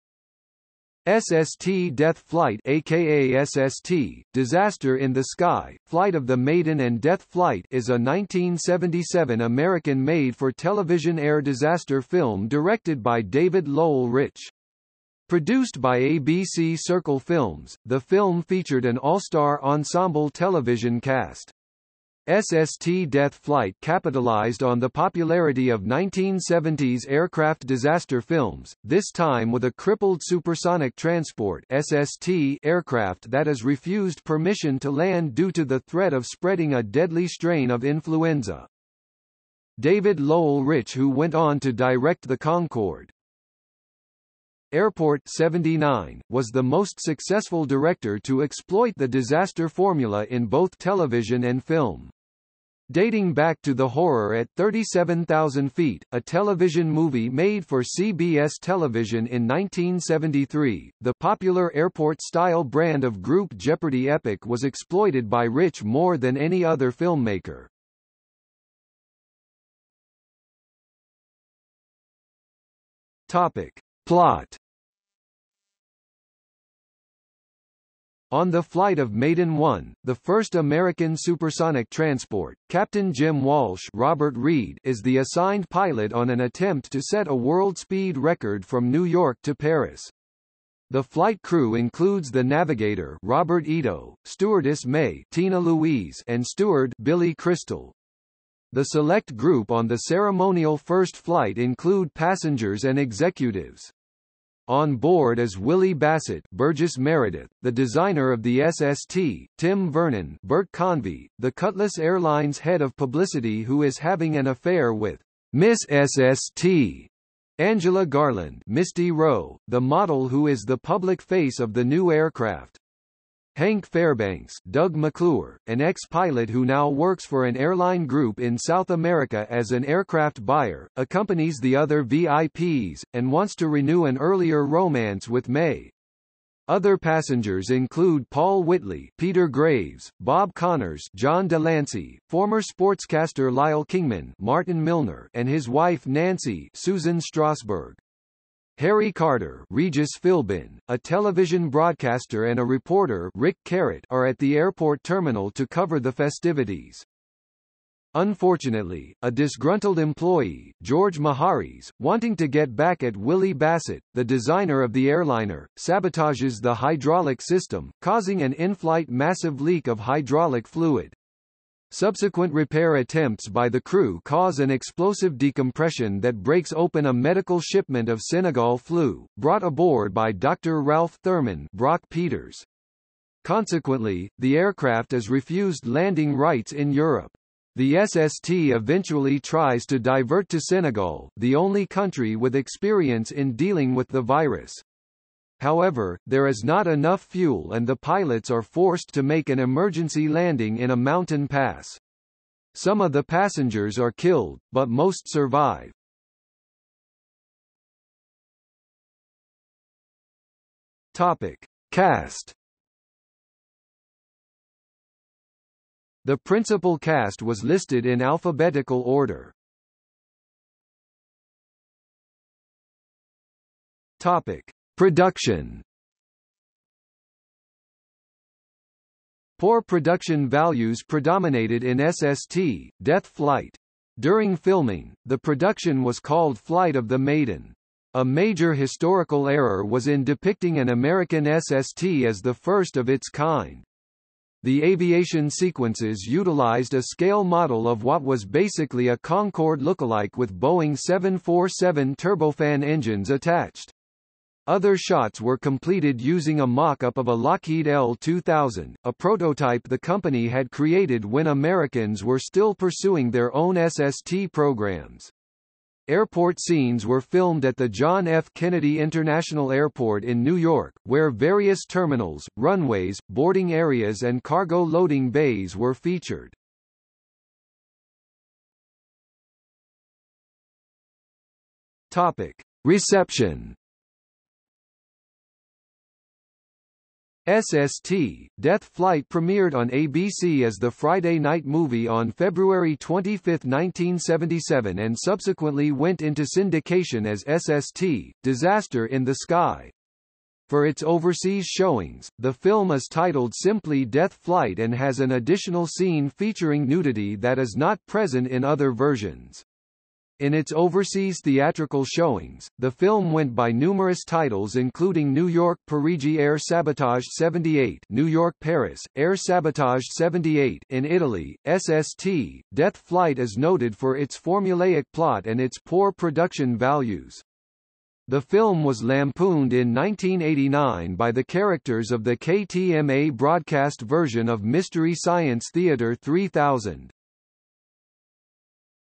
SST: Death Flight | Wikipedia audio article
00:01:09 1 Plot
00:03:27 2 Cast
00:03:40 3 Production
00:04:38 4 Reception

- Socrates

SUMMARY
SST Death Flight (aka SST: Disaster in the Sky, Flight of the Maiden and Death Flight) is a 1977 American made-for-television air disaster film directed by David Lowell Rich. Produced by ABC Circle Films, the film featured an all-star ensemble television cast. SST Death Flight capitalized on the popularity of 1970s aircraft disaster films, this time with a crippled supersonic transport (SST) aircraft that is refused permission to land due to the threat of spreading a deadly strain of influenza.
David Lowell Rich who went on to direct The Concorde ... Airport '79, was the most successful director to exploit the disaster formula in both television and film. Dating back to The Horror at 37,000 Feet, a television movie made for CBS Television in 1973, the "popular Airport-style brand of group-jeopardy epic was exploited by Rich more than any other filmmaker."